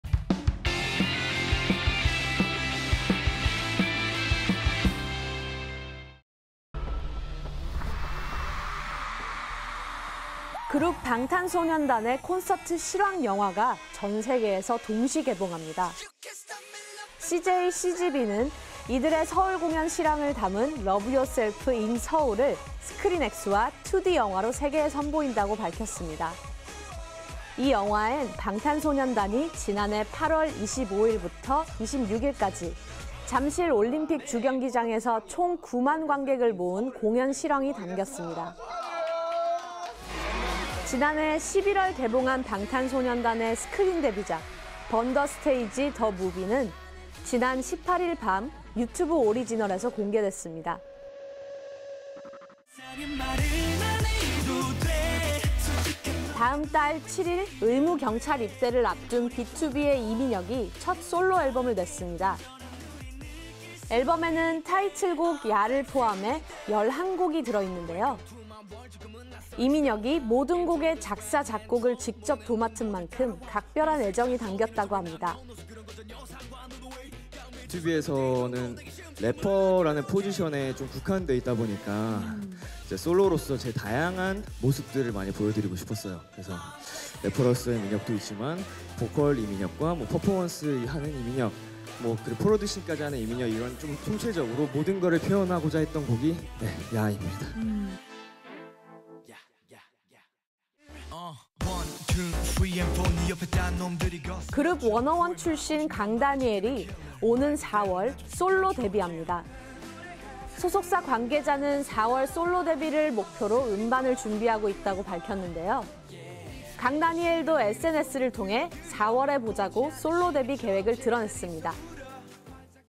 이주의 연예 이슈 - 방탄소년단, 이민혁, 강다니엘 / YTN KOREAN
방탄소년단 BTS의 콘서트를 담은 영화가 전 세계에서 동시 개봉!
다음 달 7일 의무 경찰 입대를 앞둔 비투비 BTOB 이민혁이 첫 솔로 앨범 발매!
워너원 Wanna One 출신 강다니엘이 오는 4월 솔로 데뷔!